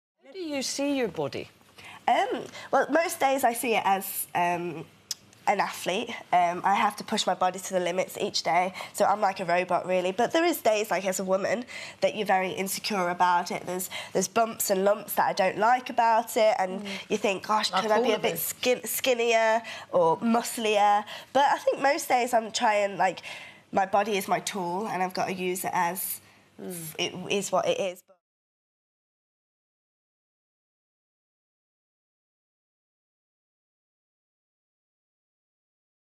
'My body is my tool' Ellie Simmonds on body confidence
YouTube channel with the last vile from the world of sport, business, entertainment, politics, etc.